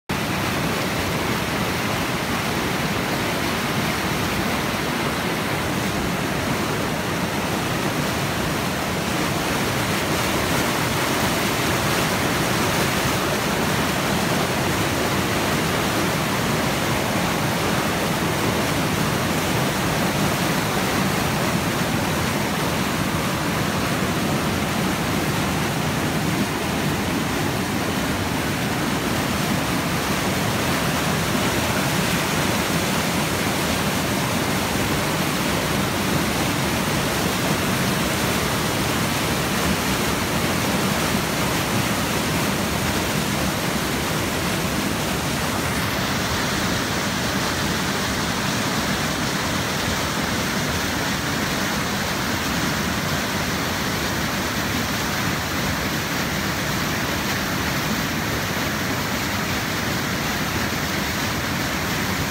Urho Kekkonen National Park - Palovankanjoki Bridge
Urho Kekkonen National Park - Palovankanjoki Bridge. Beautiful whitewater and cliffs around Palovankanjoki bridge. To see more my nature photos and videos please visit my Flickr photostream